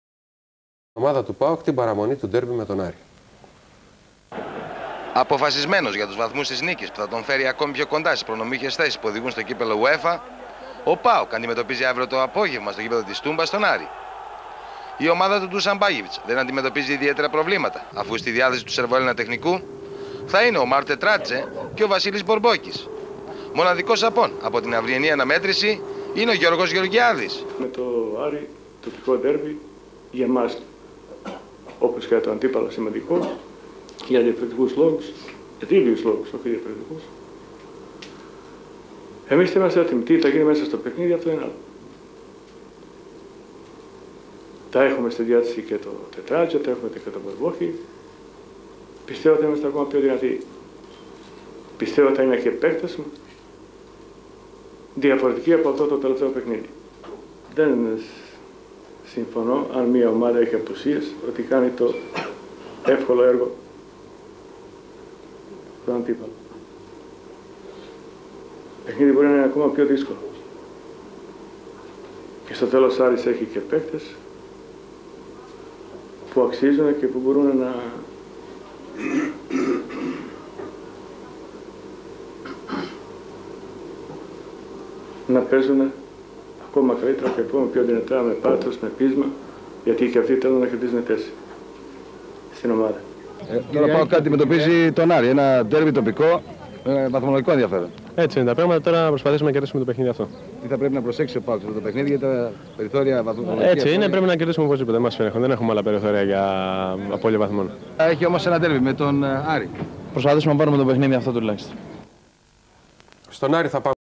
ΡΕΠΟΡΤΑΖ ΠΑΟΚ ΠΡΙΝ ΤΟ ΠΑΟΚ-ΑΡΗΣ ΤΟΥ 2000
Τα τελευταία νέα του ΠΑΟΚ πριν το παιχνίδι με τον Άρη στην Τούμπα τον Απρίλιο του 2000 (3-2).